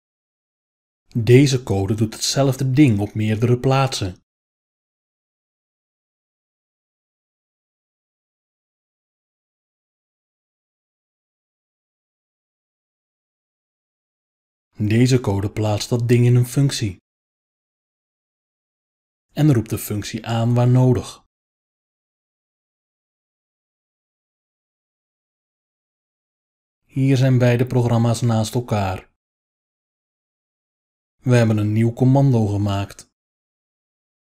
Robot Arm - Nieuwe commando's
An exercise for learning to program in Lua using Lua GUI.
Try to recreate the program shown in the video.